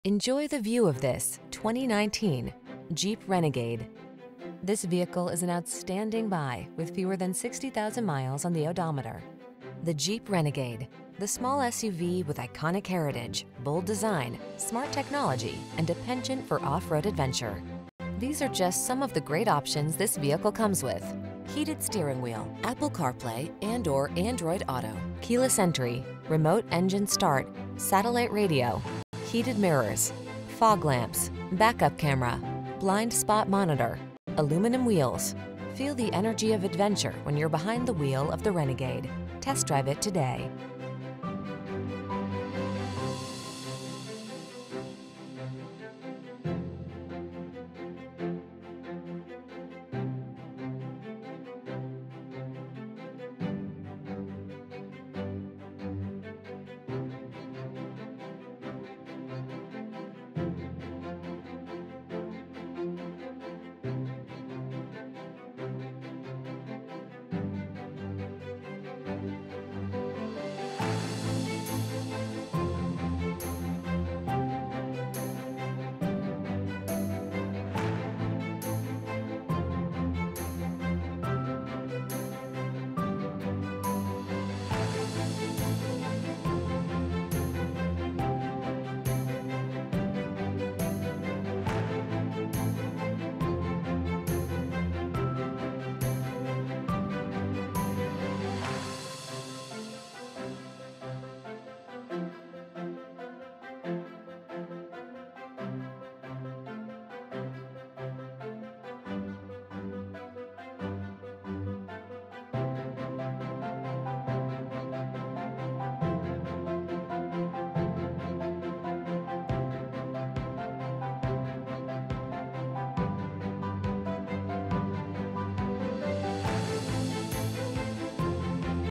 2019 Jeep Renegade Davison, Grand Blanc, Lapeer, Burton, Flint 6-32535AK
Granite Crystal Metallic Clearcoat Used 2019 Jeep Renegade Latitude available in 800 N. State Rd., Davison, Michigan at Hank Graff Chevrolet. Servicing the Davison, Flint, Grand Blanc, Lapeer, Burton, MI area.

Renegade Latitude
2019 Jeep Renegade Latitude - Stock#: 6-32535AK

800 N. State Rd

4WD 9-Speed 948TE Automatic 2.4L I4